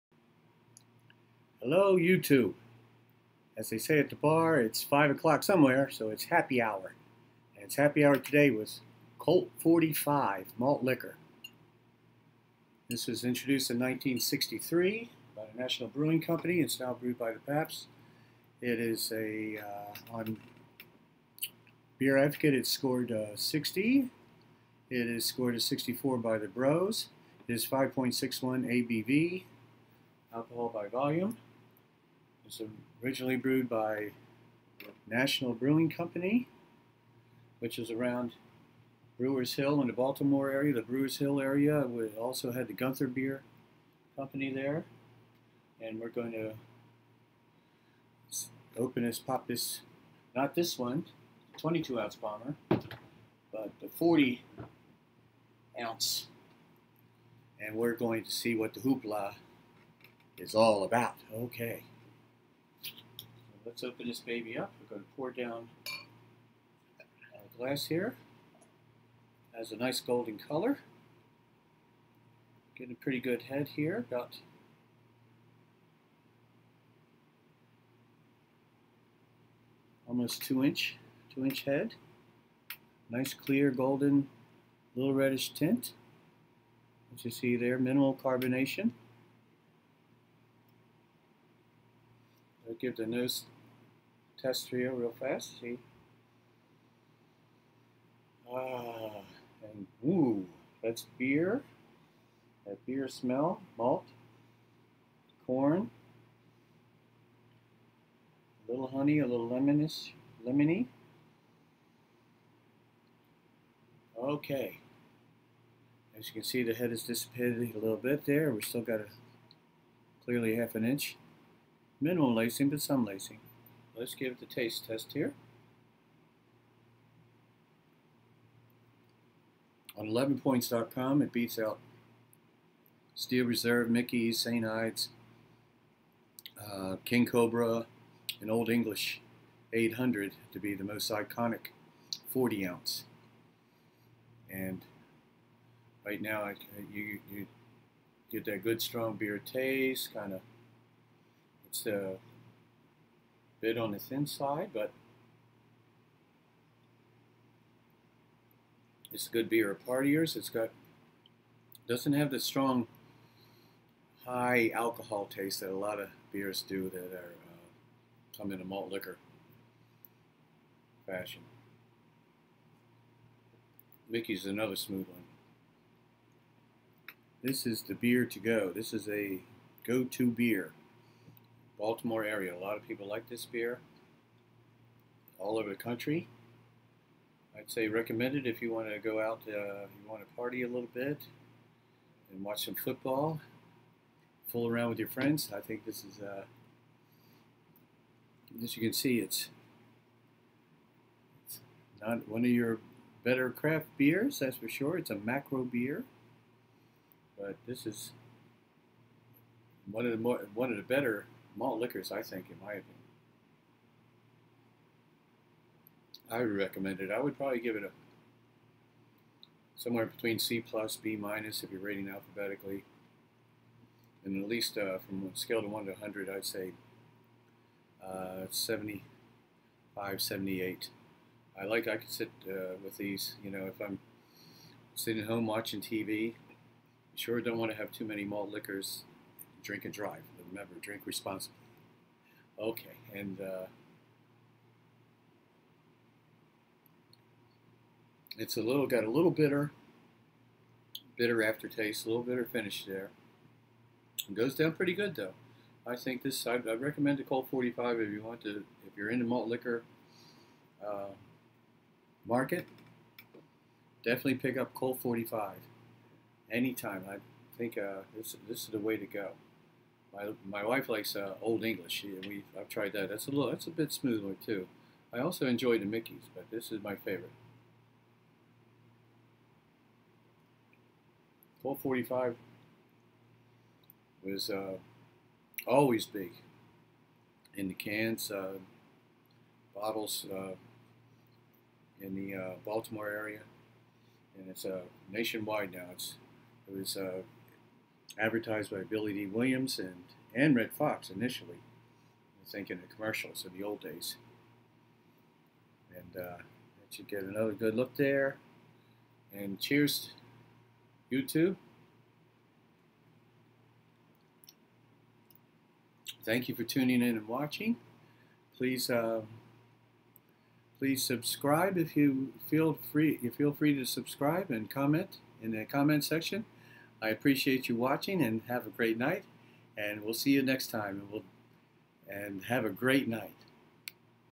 Brian's Beer Reviews # 6 Colt 45 Malt Liquor
5.61 ABV/ Introduced In 1963 by the National Brewing Co. Named after running back Jerry Hill, who was #45 for the 1963 Baltimore Colts. Actor Billy Dee Williams and Comedian Redd Foxx appeared in radio, print & TV advertisements - The Williams relationship continued for 4 decades, until he passed the torch to Snoop Dogg in 2010.